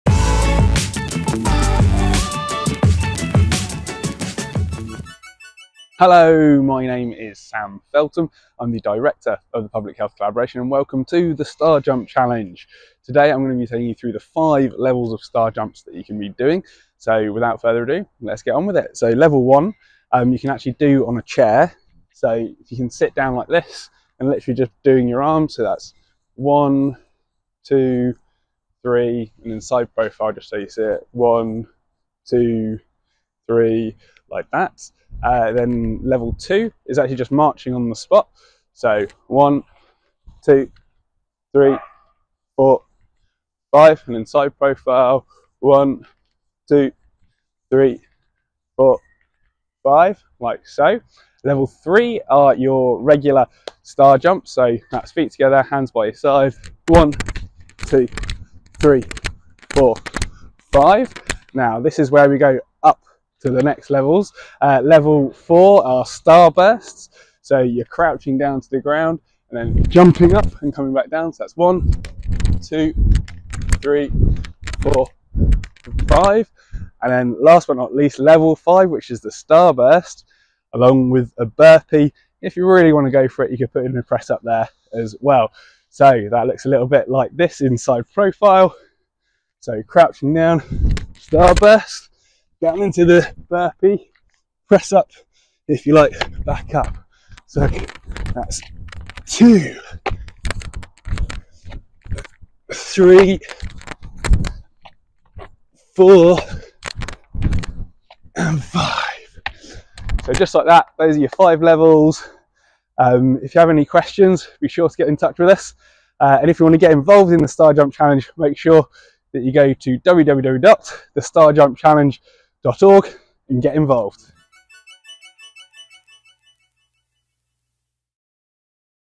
The Star Jump Challenge 2024 // The 5 levels of star jumps you can do
What is The Star Jump Challenge?
An exercise challenge to do 25 star jumps per day for 25 days in a row between 1st October and 25th October. Your star jumps can be done at home, parks, school, work or the gym.

Why you should take part?
25 people every day in the UK have an amputation due to diabetes complications. This terrifying statistic adds up to 9,360 amputees per year. To prevent this terrible tragedy from getting worse, Public Health Collaboration (Charity no. 1171887) are seeking to raise at least £9,360 to help in its efforts to reverse the type 2 diabetes epidemic.

How you can take part?
Whether you're doing the challenge as an individual or team, simply set up a JustGiving fundraiser page connected with our main campaign. Then share that page with everyone you can by posting photos and videos on social media using